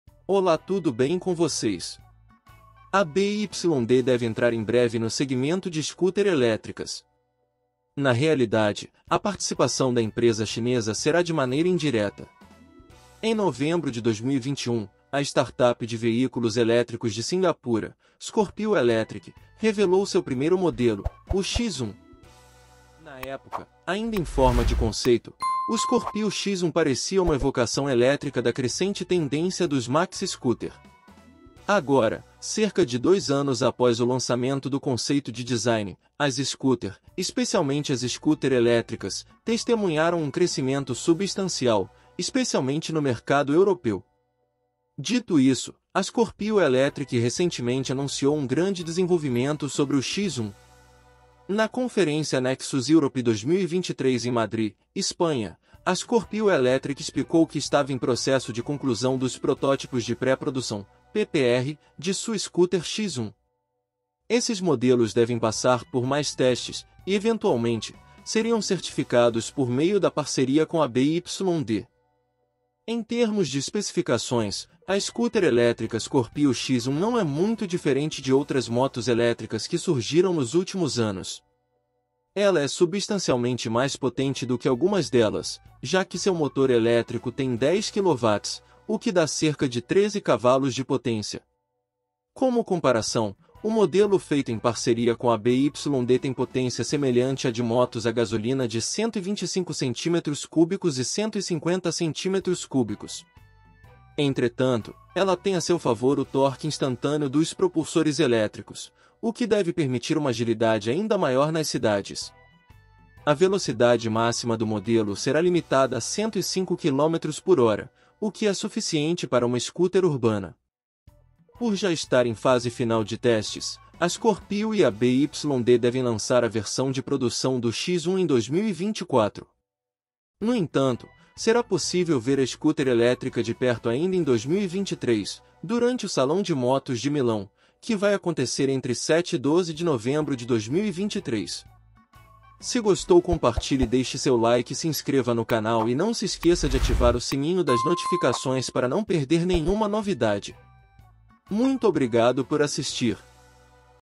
🟢 Scooter elétrica da BYD deve ser lançada em breve
ola tudo bem com voces

A BYD deve entrar em breve no segmento de scooters elétricas. Na realidade, a participação da empresa chinesa será de maneira indireta. Em novembro de 2021, a startup de veículos elétricos de Cingapura, Scorpio Electric, revelou seu primeiro modelo, o X1.
Na época, ainda em forma de conceito, o Scorpio X1 parecia uma evocação elétrica da crescente tendência dos maxi-scooters. Agora, cerca de dois anos após o lançamento do conceito de design, as scooters – especialmente as scooters elétricas – testemunharam um crescimento substancial, especialmente no mercado europeu.

Dito isso, a Scorpio Electric recentemente anunciou um grande desenvolvimento sobre o X1. Na Conferência Nexus Europe 2023 em Madri, Espanha, a Scorpio Electric explicou que estava em processo de conclusão dos Protótipos de Pré-Produção (PPR) de sua scooter X1. Esses modelos devem passar por mais testes e, eventualmente, seriam certificados por meio da parceria com a BYD.
Em termos de especificações, a scooter elétrica Scorpio X1 não é muito diferente de outras motos elétricas que surgiram nos últimos anos. Ela é substancialmente mais potente do que algumas delas, já que seu motor elétrico tem 10 kW, o que dá cerca de 13 cavalos de potência.

Como comparação, o modelo feito em parceria com a BYD tem potência semelhante a de motos a gasolina de 125 cc e 150 cc. Entretanto, ela tem a seu favor o torque instantâneo dos propulsores elétricos, o que deve permitir uma agilidade ainda maior nas cidades. A velocidade máxima do modelo será limitada a 105 km/h, o que é suficiente para uma scooter urbana.
Por já estar em fase final de testes, a Scorpio e a BYD devem lançar a versão de produção do X1 em 2024. No entanto, será possível ver a scooter elétrica de perto ainda em 2023, durante o Salão de Motos de Milão, que vai acontecer entre 7 e 12 de novembro de 2023.